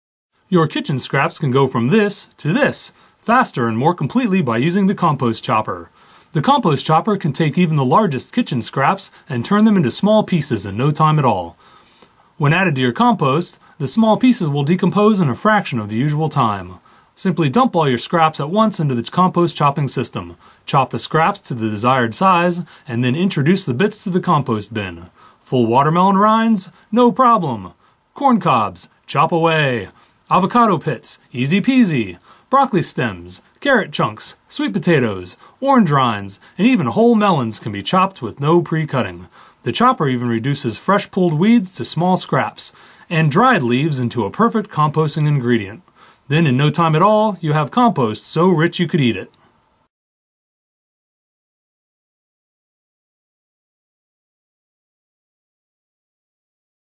The Compost Chopper Pulverizes Vegetable Scraps
The compost chopper is a four-bladed tool that works with a circular chopping board designed to fit within a standard five-gallon bucket.  Scraps are tossed into the bucket and chopped between the board and the chopper.   By creating uniform, smaller pieces, composting time is greatly reduced and the quality of the compost is increased.